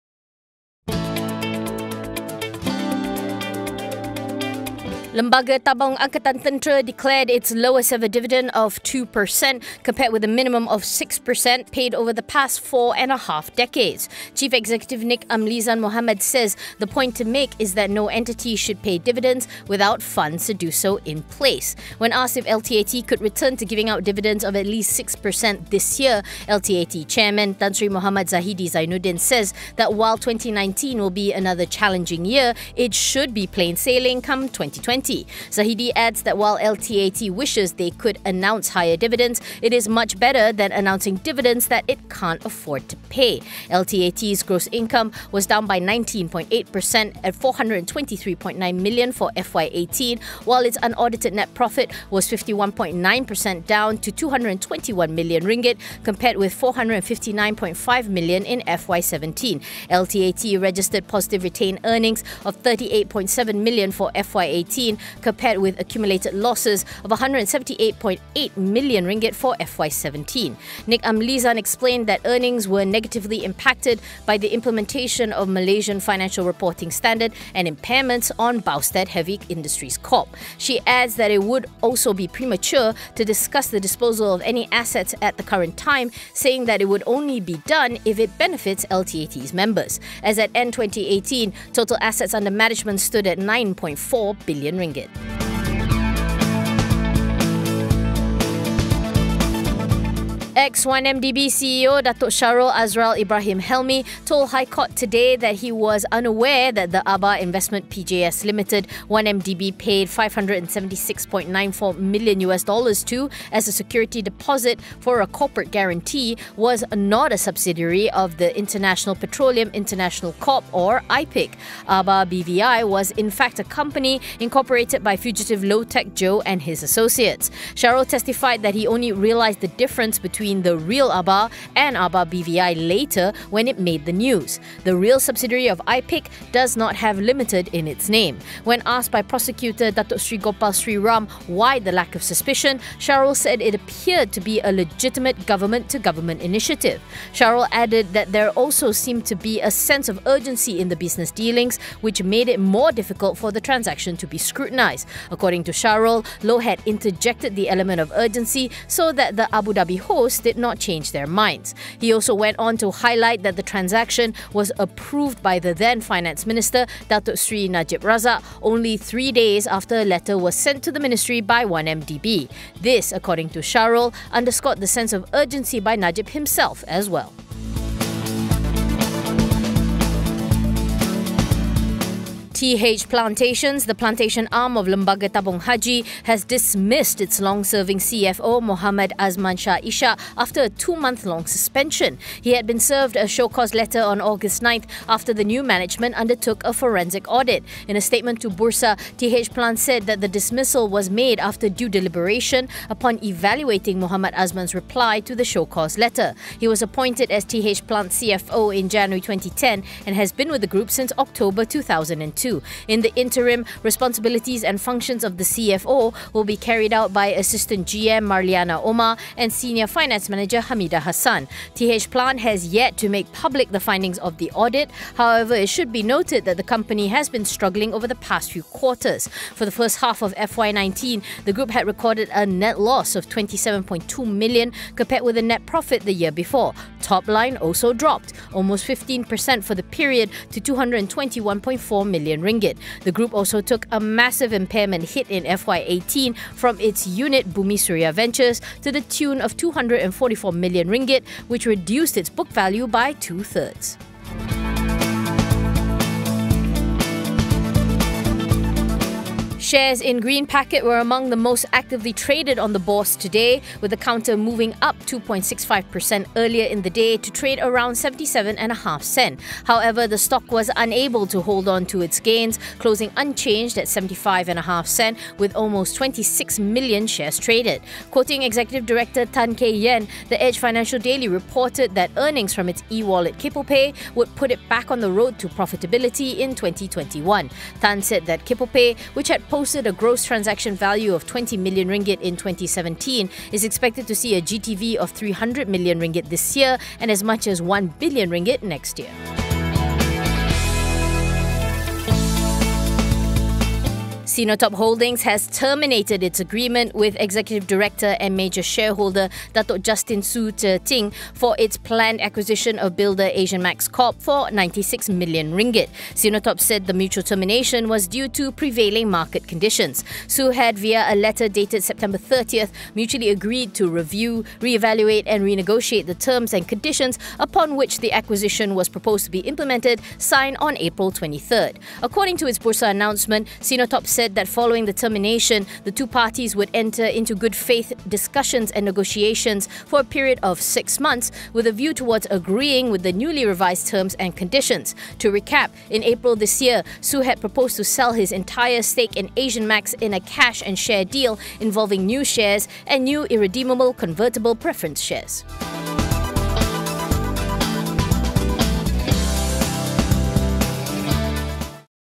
EVENING 5: LTAT delivers lowest dividend in decades
Lembaga Tabung Angkatan Tentera has delivered its lowest dividend in decades, days after an investigative audit. Meanwhile, TH Plantations has decided to dismiss its CFO of nine years.